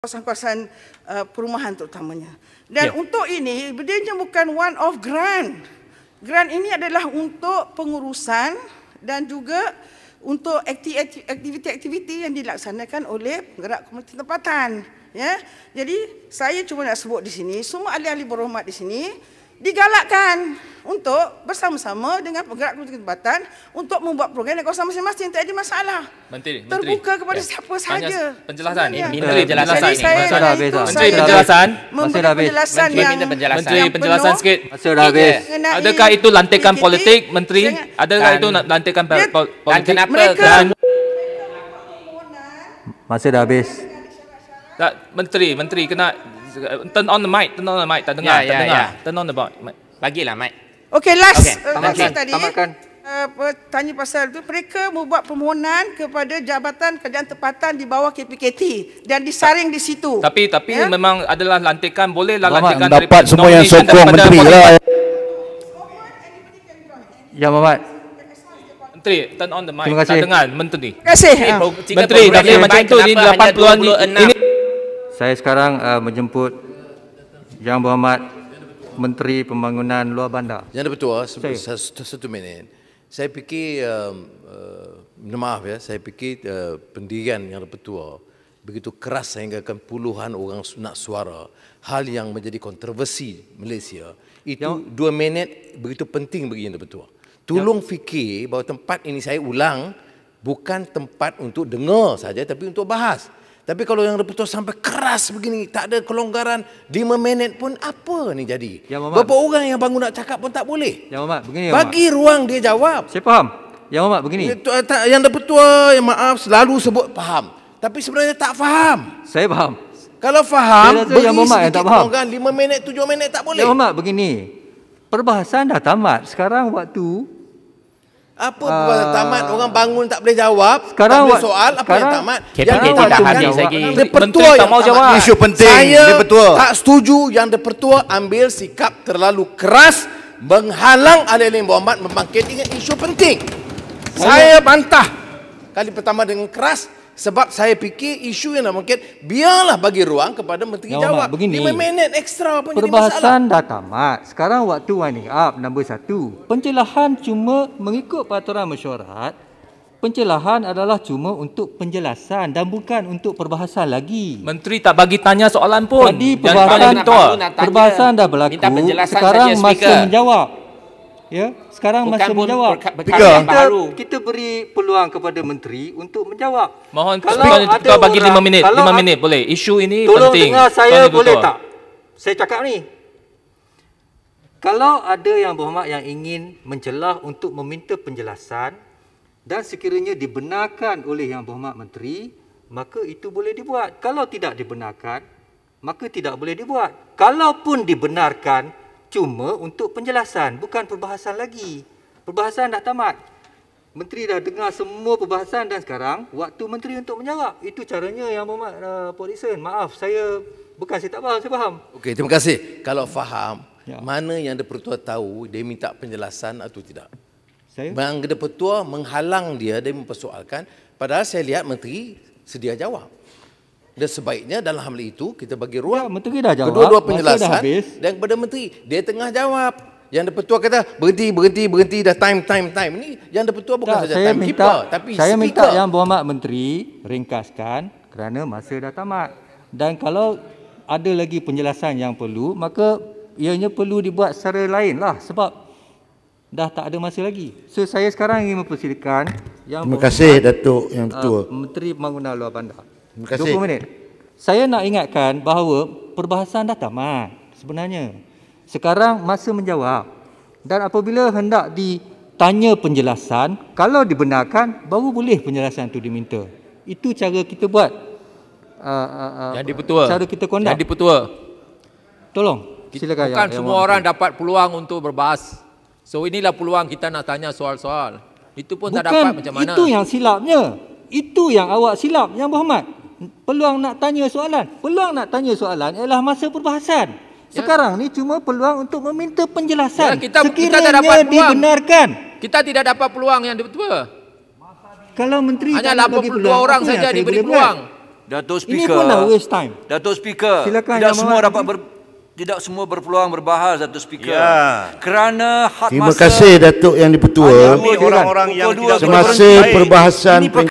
Speaker Halang Hujah Balas MP [25.11.2020]
Saya bantah sikap Speaker Dewan Rakyat yang menghalang ahli-ahli Parlimen bangkitkan isu penting dan kontroversi melibatkan peruntukan begitu besar kepada sebuah NGO ini.

Saya yakin menteri berkenaan sedia menjawab tetapi dihalang Speaker meski untuk 5 minit.

Sikap ini tidak meletakkan institusi Parlimen di tempat yang sepatutnya.